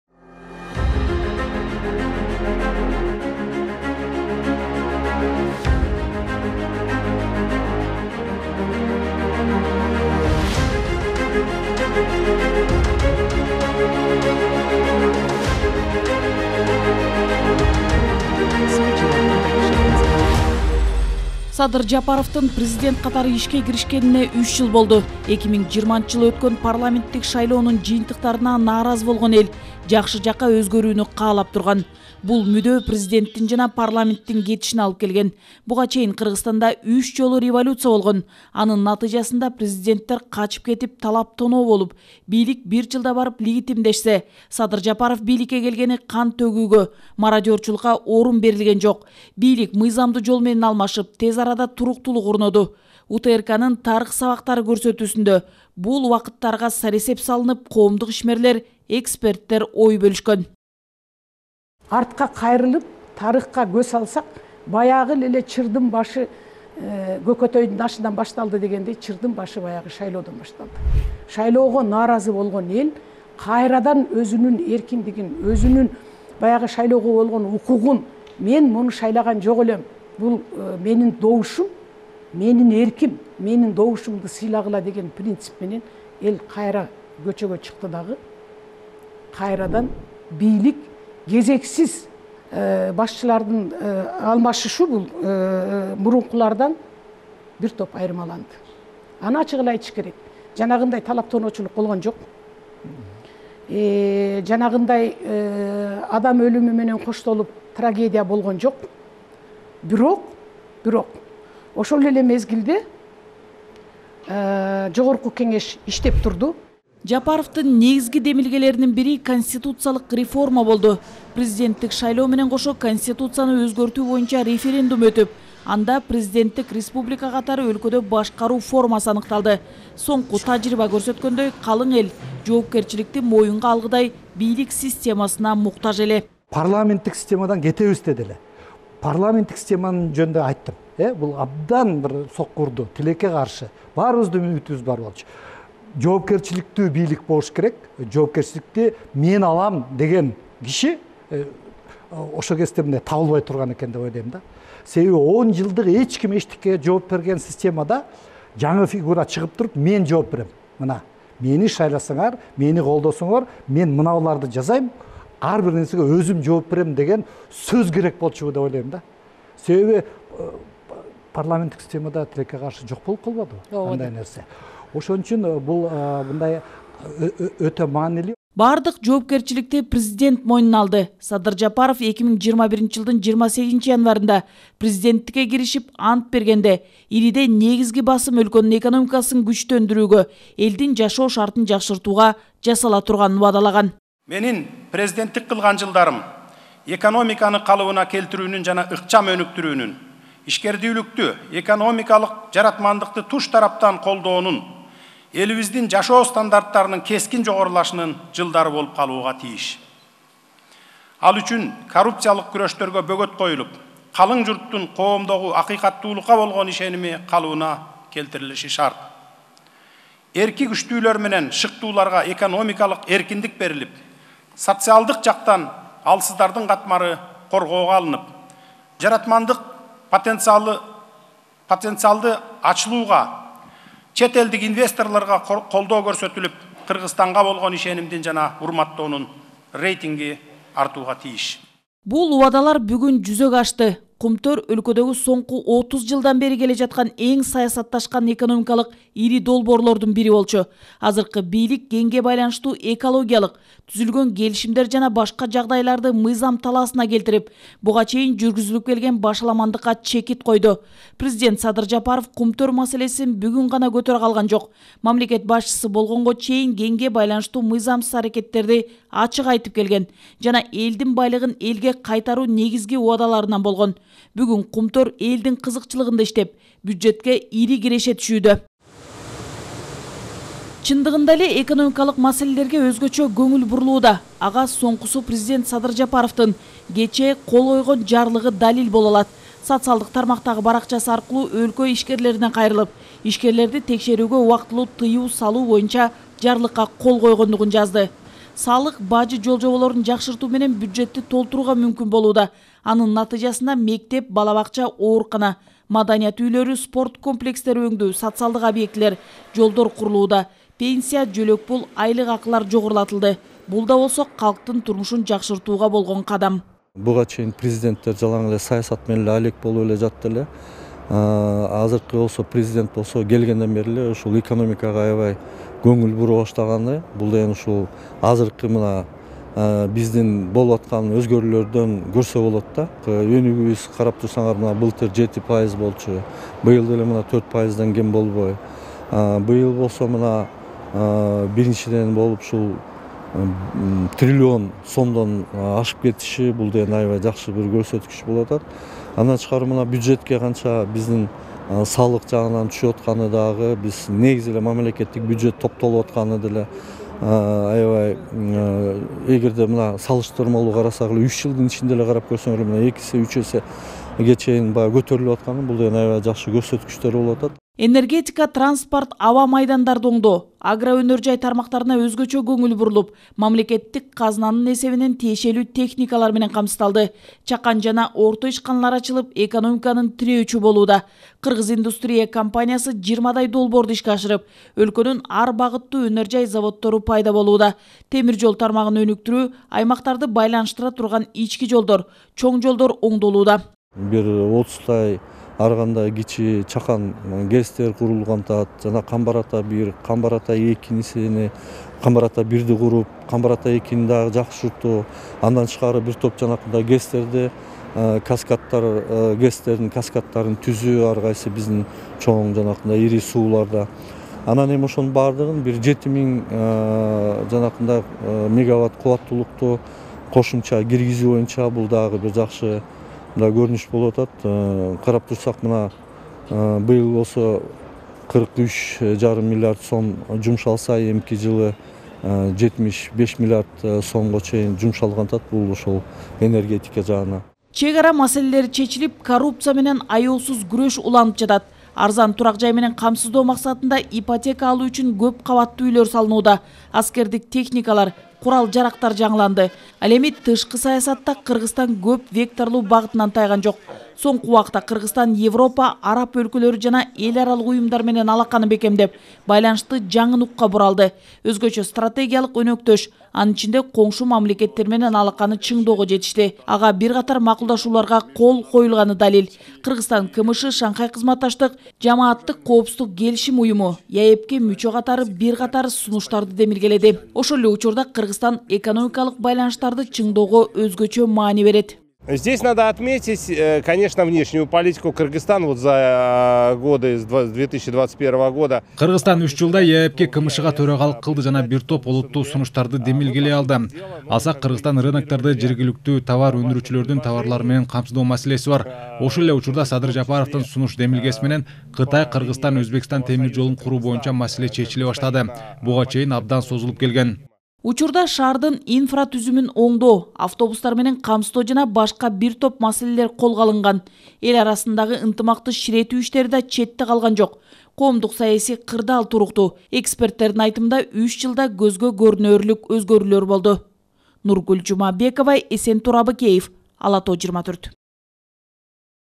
Садыр Жапаровдун президенттик кызматтагы үч жылында кандай өзгөрүүлөр болду?
Бүгүн Садыр Жапаровдун президент болгонуна үч жыл болду. 2021-жылдын 10-январында мөөнөтүнөн мурда Президенттик шайлоо өтүп, эл мамлекет башчылыгына Садыр Жапаровду шайлады. Ушул эле күнү Конституцияны өзгөртүү боюнча референдум өтүп, анда президенттик республика катары өлкөдө башкаруу формасы аныкталган. Ошол күндөн тартып өлкөбүздүн жаңы тарыхында маанилүү этап - мамлекеттин экономикалык, социалдык-саясий системаны жаңылоо, мамлекеттик бийликте жана коомдо укуктук тартипти орнотуу башталды.